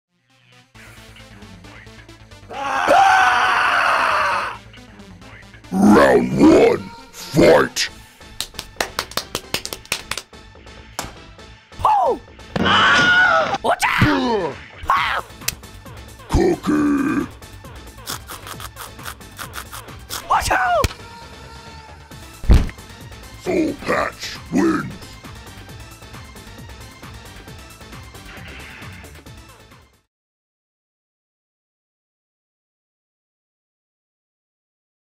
Mortal Kombat
This video was produced by students to demonstrate audio SFX/Foley effects in Doyle's Movie making course.